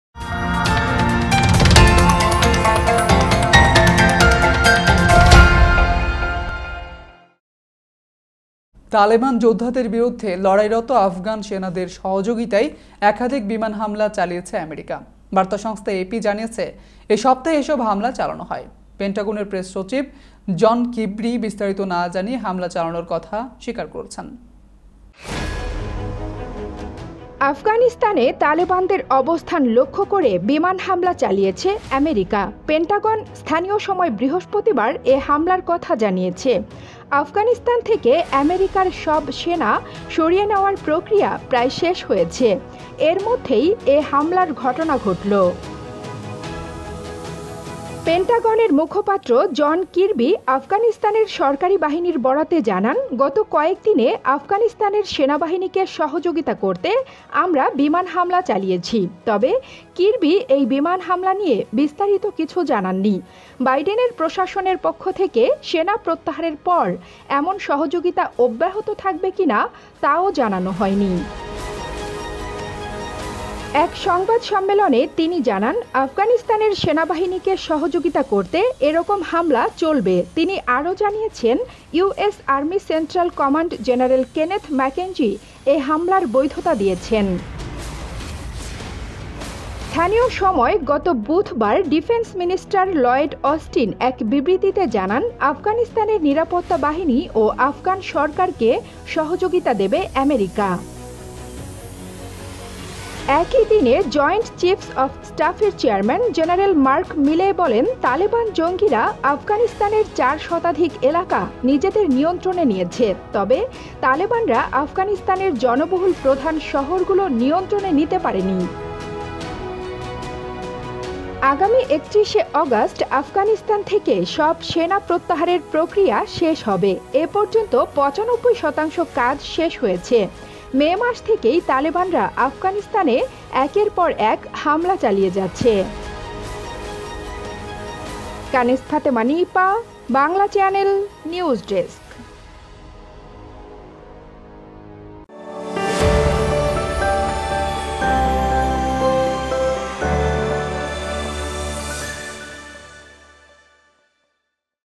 আফগানিস্তানে বিমান হামলা চালিয়েছে অ্যামেরিকা
🌟 বিস্তারিত: তালেবান যোদ্ধাদের বিরুদ্ধে লড়াইরত আফগান সেনাদের সহযোগিতায় একাধিক বিমান হামলা চালিয়েছে অ্যামেরিকা। বার্তা সংস্থা এপি জানিয়েছে, এই সপ্তাহে এসব হামলা চালানো হয়। পেন্টাগনের প্রেস সচিব জন কিরবি বিস্তারিত না জানিয়ে হামলা চালানোর কথা স্বীকার করেছেন।

🌟  যোগাযোগের ঠিকানা:
Bangla Channel